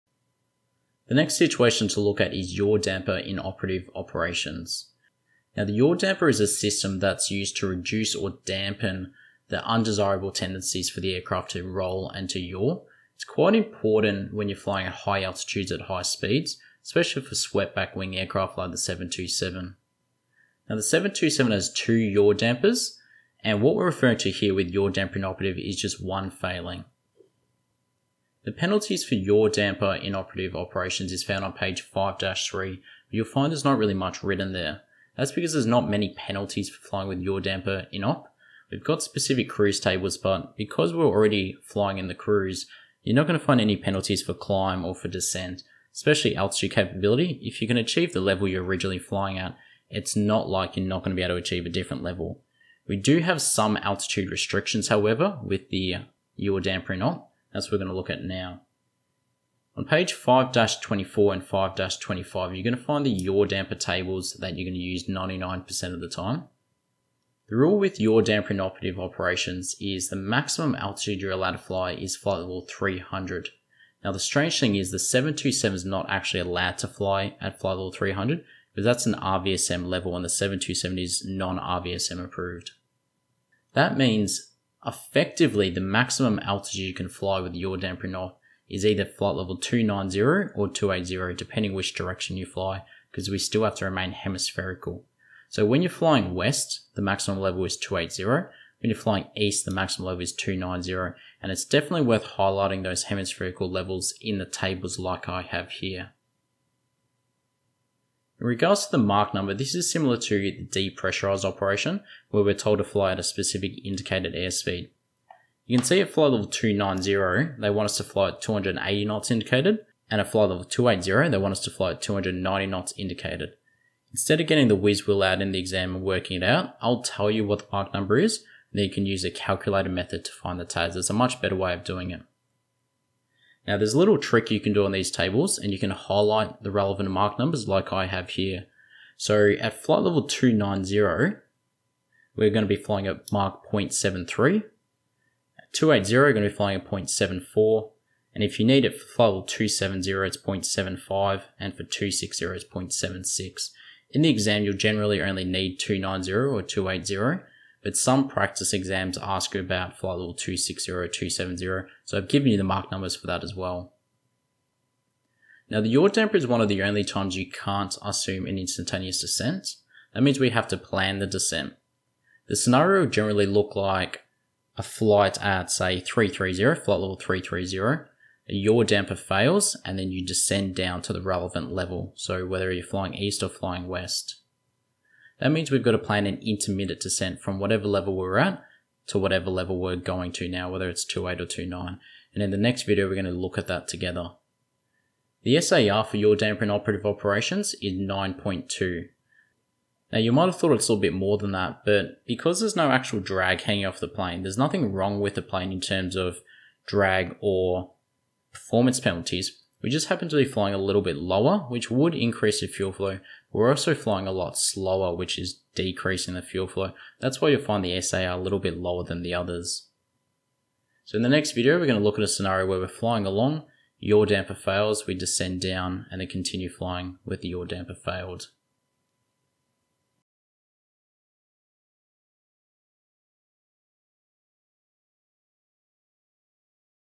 YD INOP Explanation for CASA ATPL Flight Planning
This is a small extract from our Online ATPL Flight Planning course. In this video, we cover an introduction to YD INOP flight plans.

To get more information or to access the entire ATPL Flight Planning course go